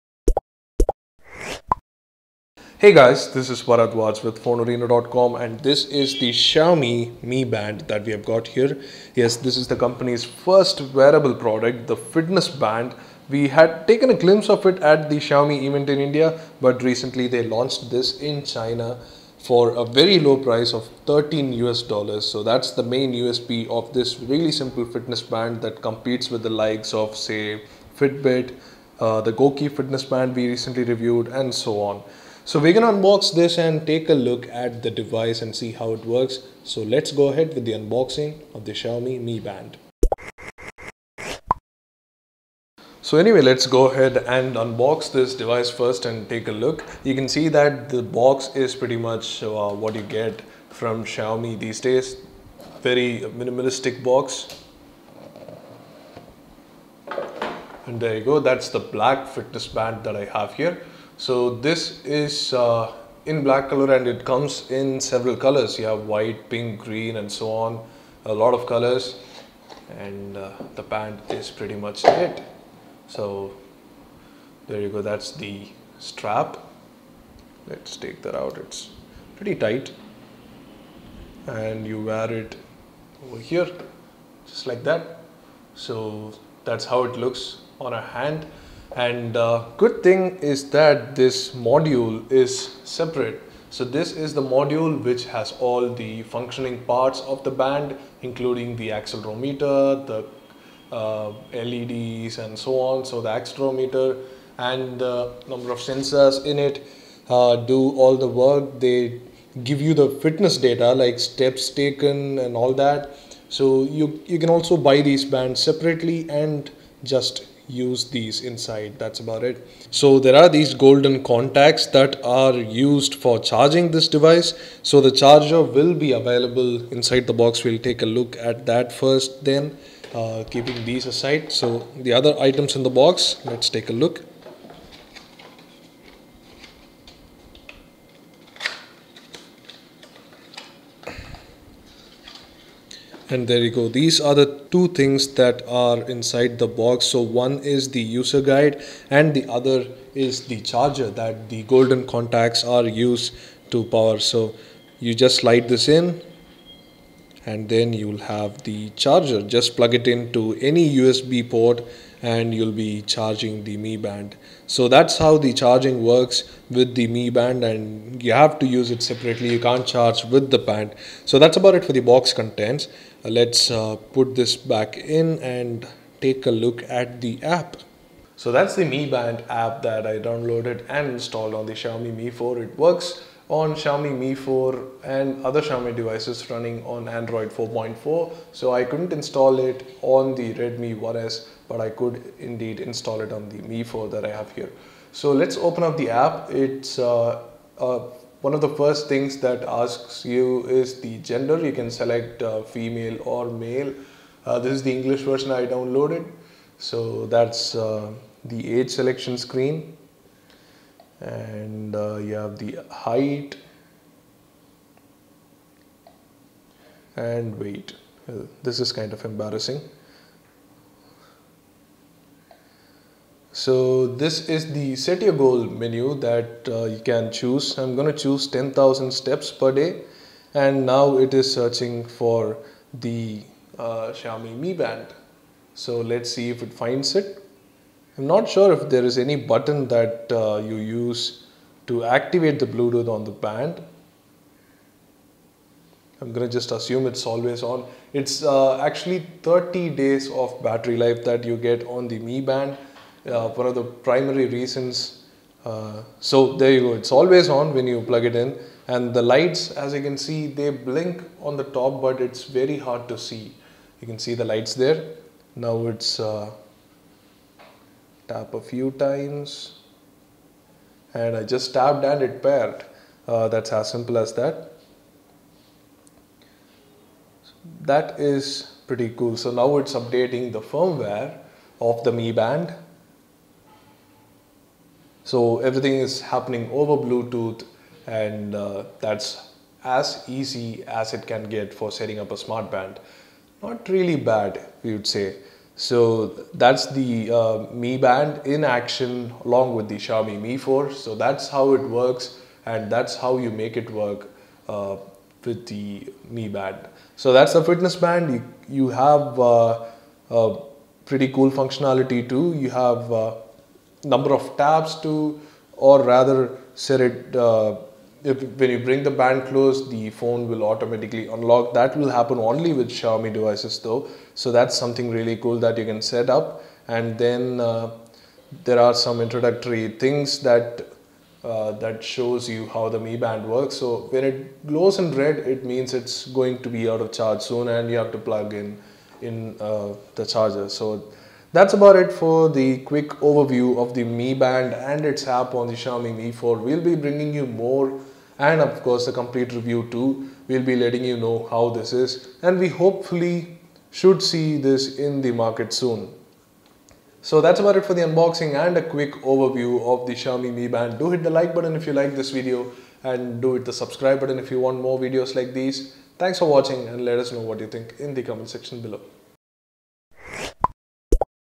Xiaomi MiBand Unboxing, Setup and First Impressions
Xiaomi MiBand Unboxing by FoneArena. unboxes the Xiaomi MiBand. Xiaomi MiBand has three LED notification lights and connects to your Android smartphones running Android via Bluetooth 4.0 low energy (BLE). It has a set of sensors that monitors your fitness and tracks your sleep. The data can be share to social networks easily with the companion app that would also offer you health advice. It has aluminum surface and skin-friendly wristband.